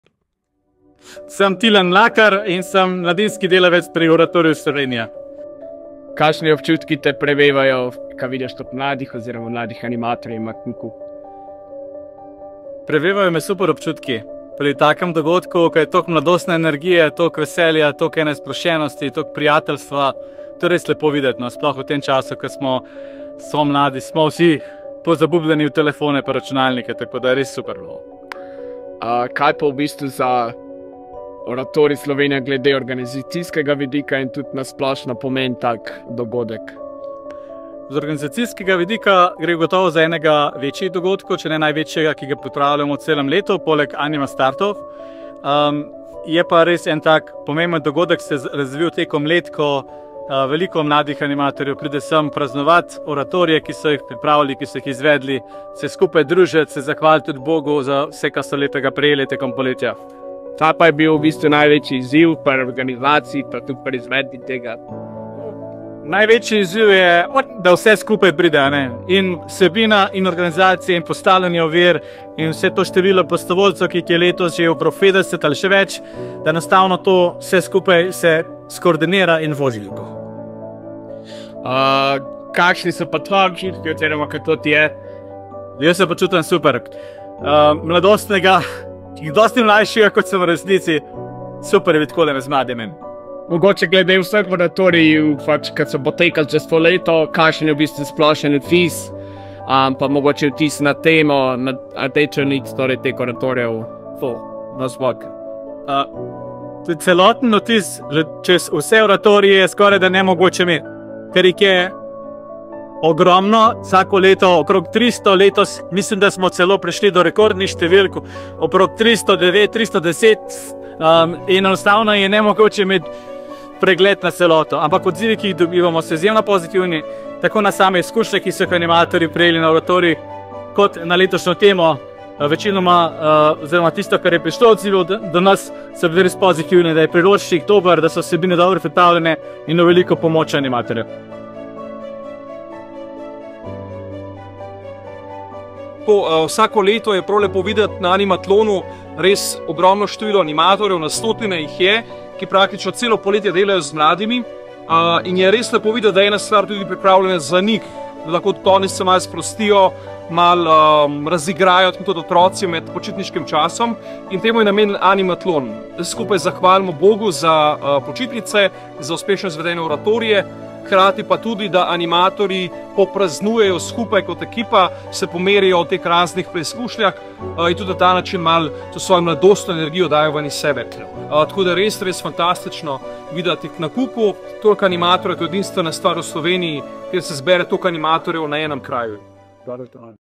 Tilen Mlakar o Animatlonu in oratorijih
Animatlon je imenitna prireditev, oviratlon za animatorje. O njem sta spregovorila Tilen Mlakar in Gašper Otrin.